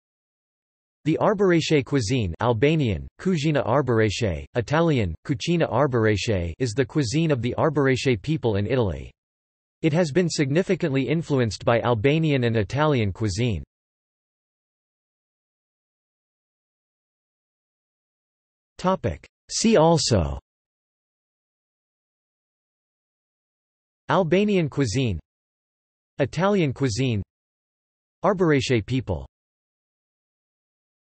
Arbëreshë cuisine | Wikipedia audio article
This is an audio version of the Wikipedia Article:
Arbëreshë cuisine

00:00:15 undefined

- Socrates

SUMMARY
The Arbëreshë cuisine (Albanian: Kuzhina Arbëreshë; Italian: Cucina Arbëreshë) is the cuisine of the Arbëreshë people in Italy. It has been significantly influenced by Albanian and Italian cuisine.